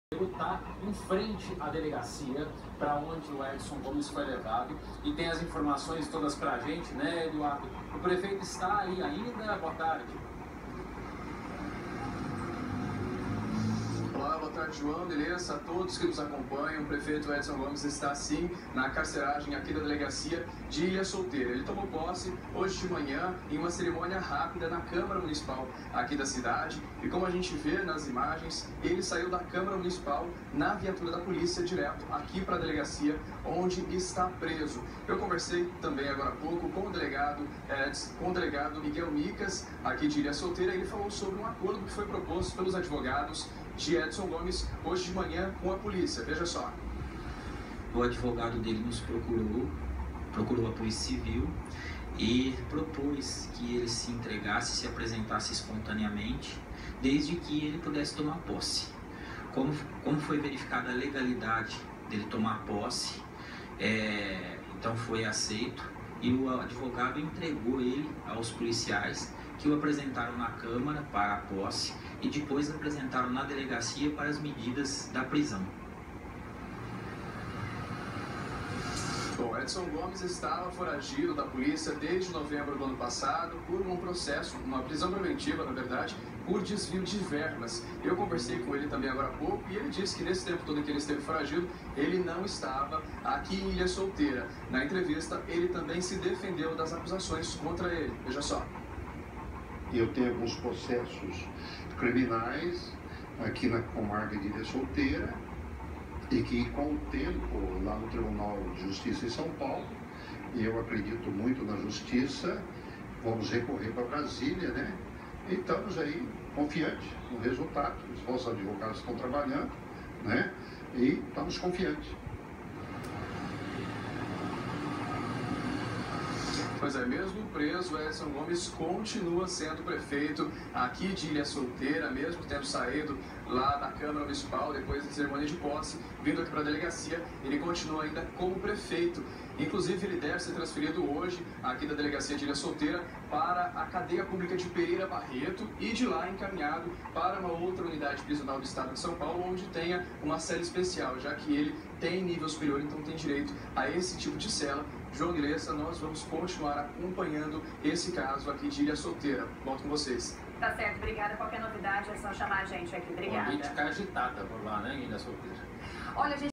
Prefeito de ilha solteira Edson gomes foi preso
Prefeito da ilha solteira tomou posse e foi preso em 2017 em seu novo mandato.